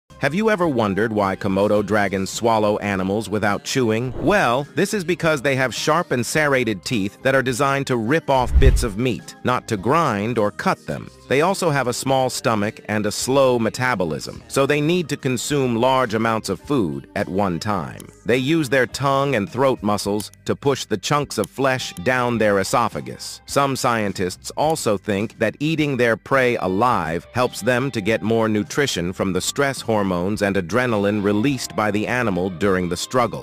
Why Komodo Dragons Don’t Chew Their Food?🤔🐲
Komodo dragons are the world’s largest living lizards, and they have a unique way of eating their food. Instead of chewing their prey 😕into smaller pieces, they swallow it whole, using their powerful jaws and serrated teeth. But why do they do this, and what are the advantages of this feeding habit? In this video, we will explore the reasons behind the Komodo dragon 🐉